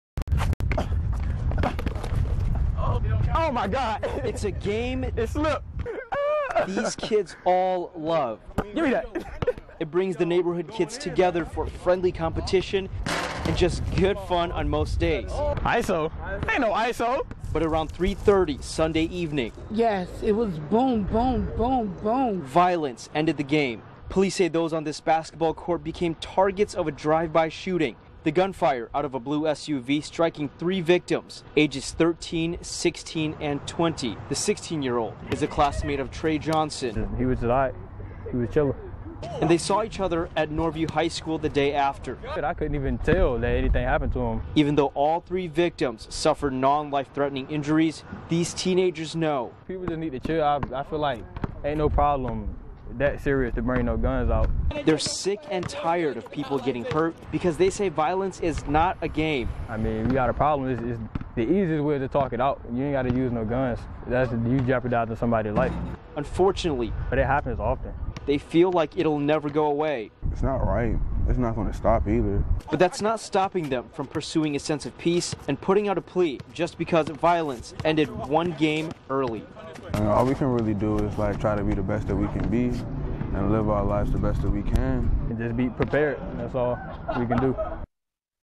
"Live the best you can live"
Teens speak out about violence after a drive-by shooting hurt three people at a neighborhood basketball court.